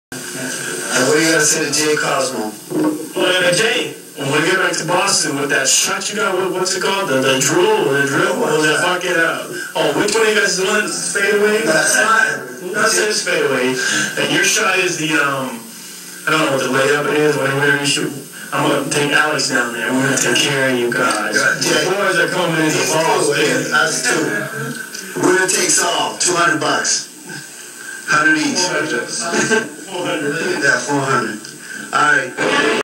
Alberto Vasallo
Miami 1994 - Jose Canseco and Alex Rodriguez challenge Alberto Vasallo and Jay Cosmo to hoops game for $400 - winners take all!